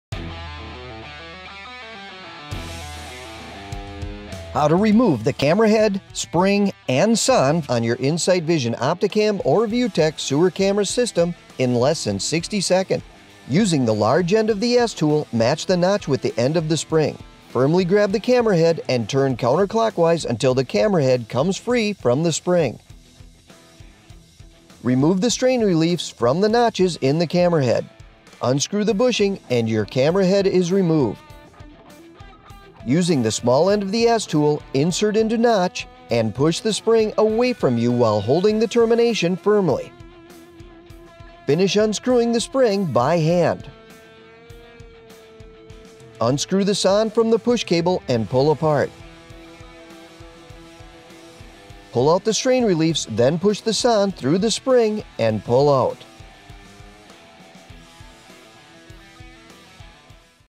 How to Remove Camera Head, Spring and Sonde - Insight | Vision
How to remove the Camera Head, Spring and Sonde on your Insight Vision Opticam or Vutek sewer camera system in less than 60 seconds
Using the large end of the S-Tool, match the notch with the end of the spring, firmly grab the camera head and turn counterclockwise until the camera head comes free from the spring.
Remove the strain reliefs from the notches in the camera head. Unscrew the bushing and your camera head is removed.
Using the small end of the S-Tool, insert into notch and push the spring away from you while holding the termination firmly.
Finish unscrewing the spring by hand.
Unscrew the sonde from the push cable and pull apart. Pull out the strain reliefs then push the sonde through the spring and pull out.
Insight | Vision is an advanced inspection manufacturer for many industries but mainly for pipes, drains and sewers lines.